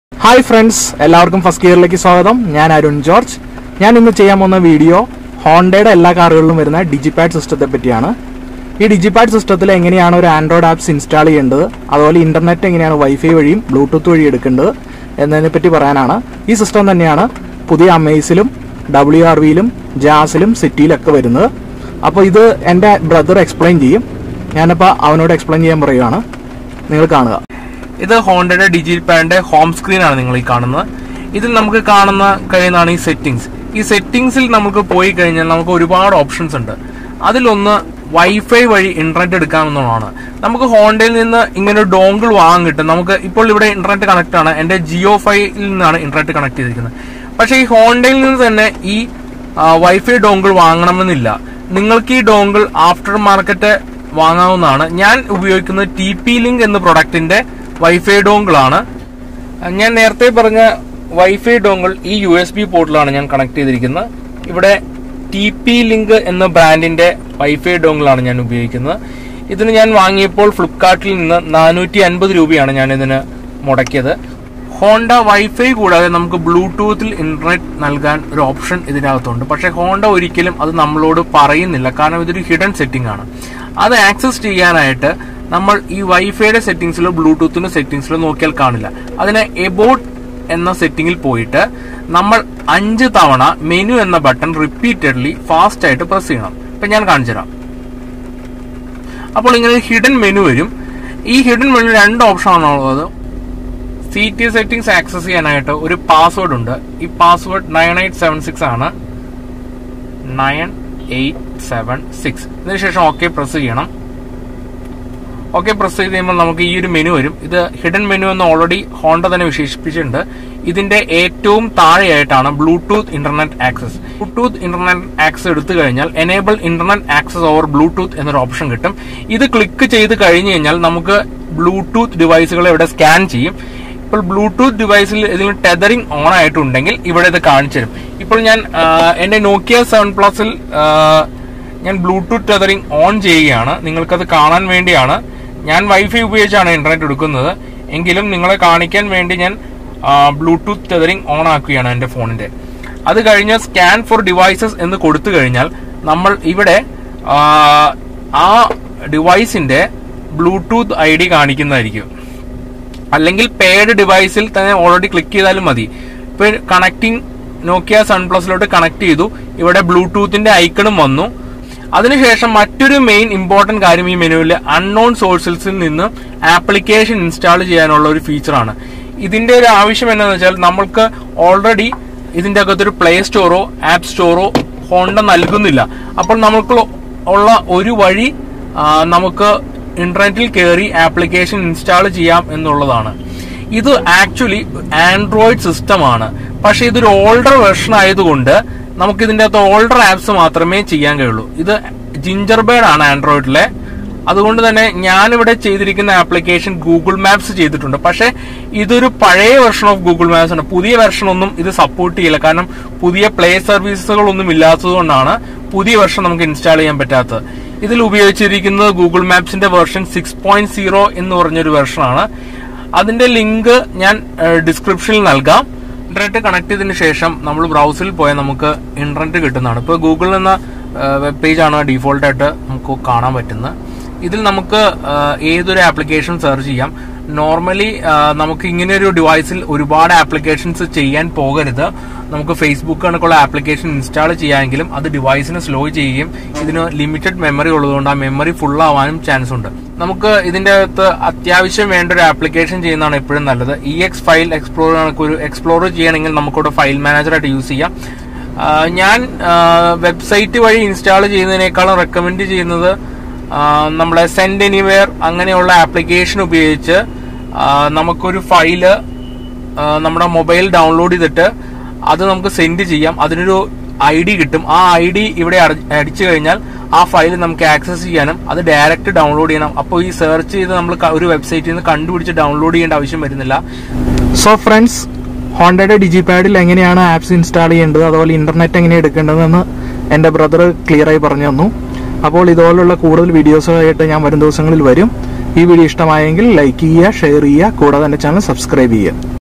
Internet in honda amaze,wrv,city,jazz & install google maps and android apps, Arun George vlogs
This video describes how to connect internet in honda cars and how to install Google maps and Android apps.

Link to install google map.

TP link wifi dongle link to buy.